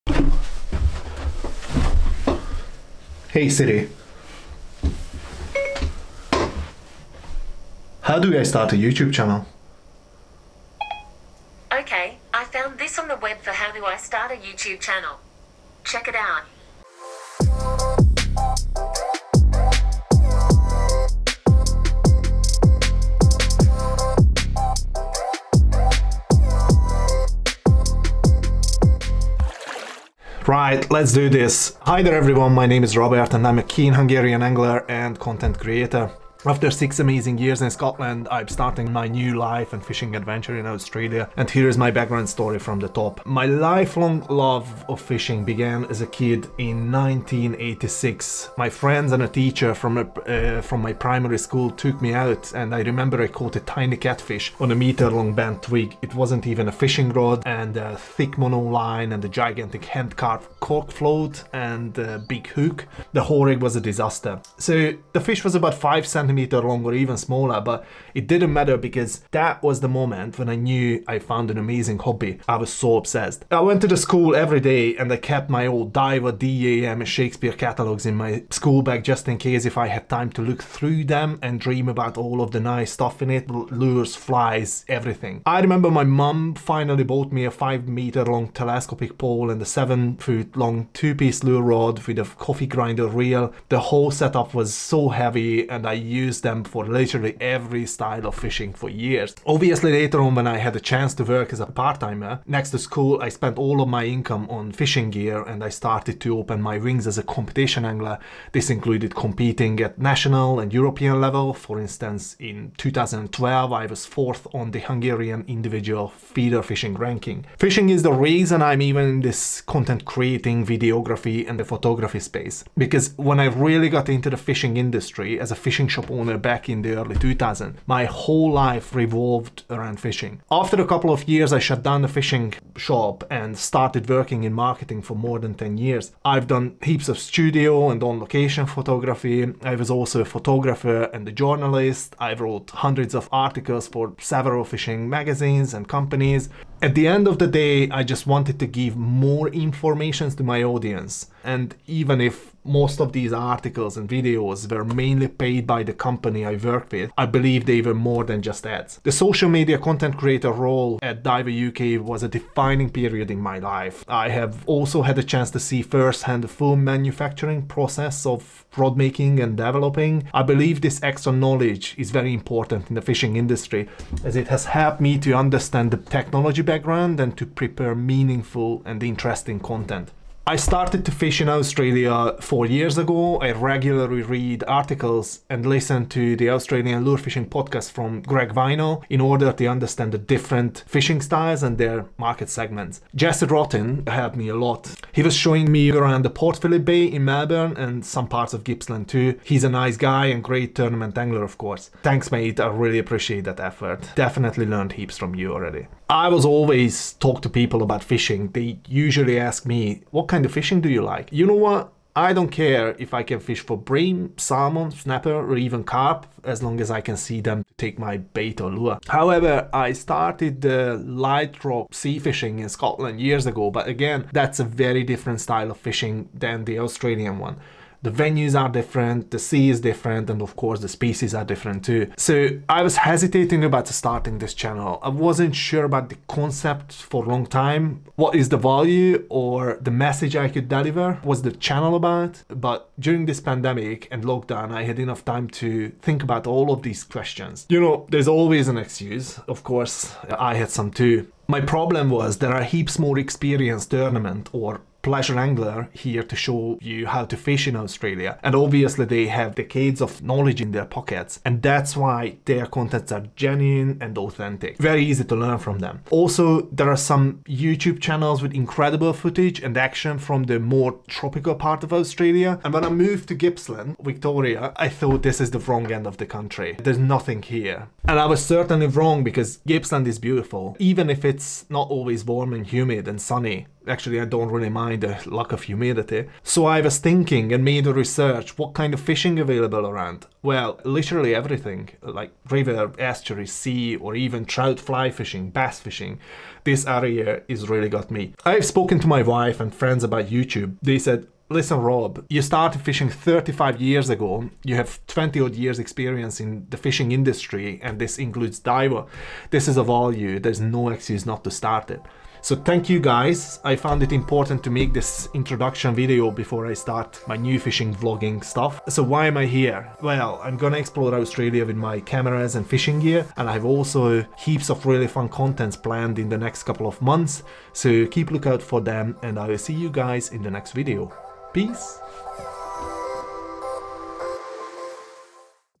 THE BEGINNING OF MY NEW FISHING ADVENTURE | Why I am Here | S01EP01
Fishing | Filming | Adventures

That’s my plan for the future! Finally I had enough time to set up my studio and start this YouTube Fishing Vlog, that I had always really wanted to do but always found an excuse to not do it.

I’m going to explore Australia with my cameras and fishing gear, I have heaps of really fun content planned in the next couple of months, so keep a lookout for them.

I would love your support, please give me any feedback you may have. ✌️